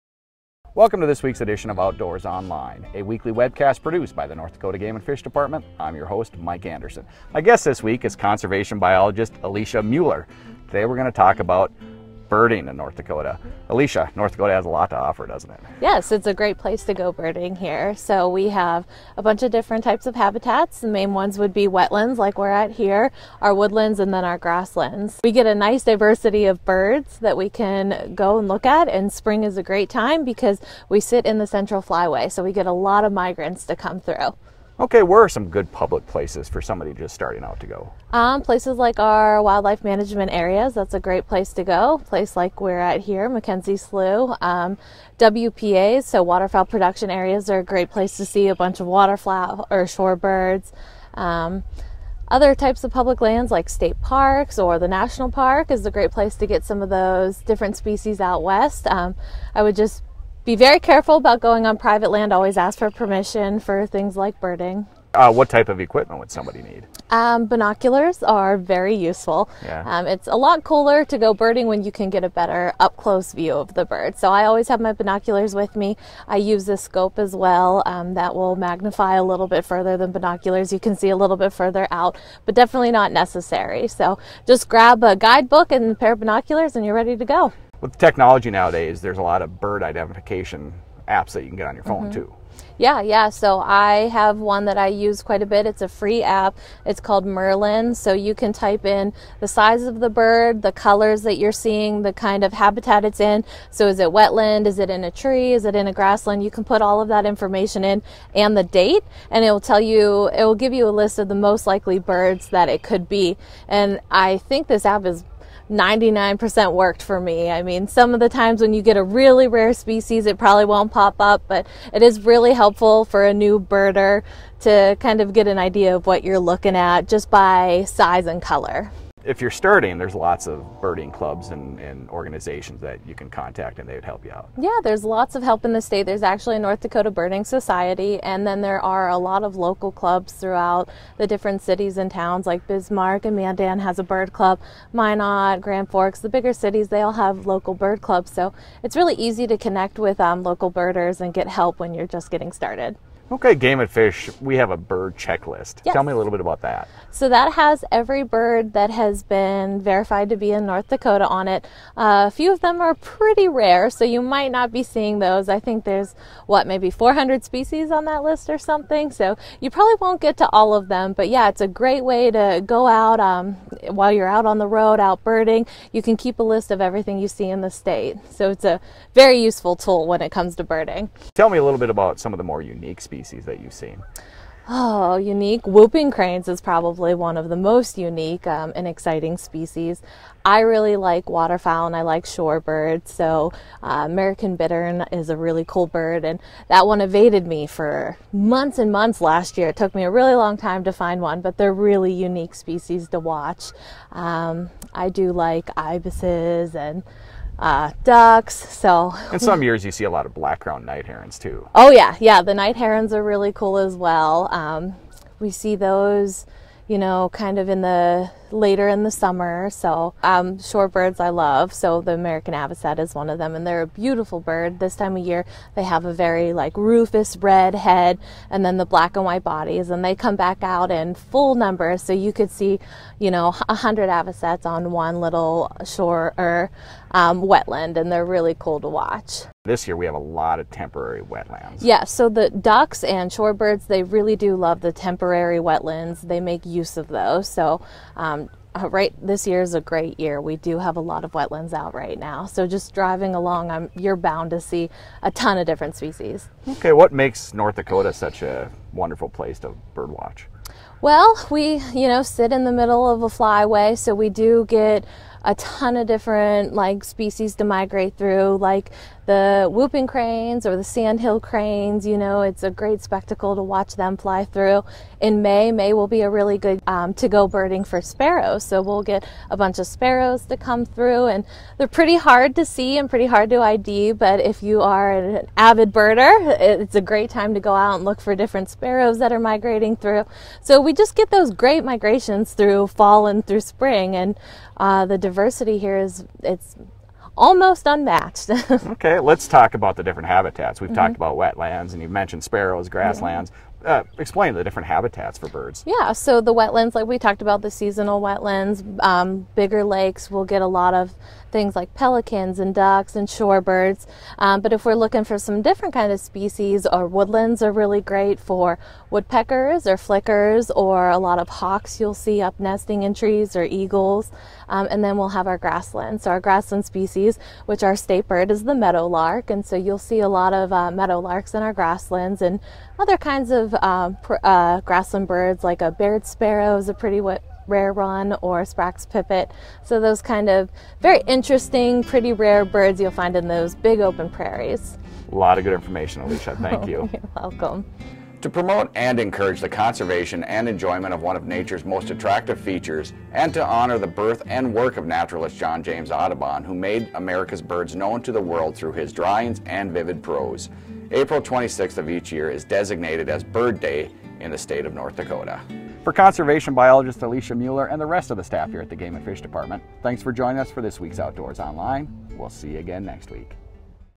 Bird Watching in North Dakota - NDGNF - 04-25-19
Are you looking for a fun outdoor pastime as the weather warms? Consider birding. North Dakota sits in the Central Flyway so the state plays temporary host to many migrants in the spring and fall. It also provides nesting habitat for many different grassland, water and woodland bird species. Learn more in this week's webcast.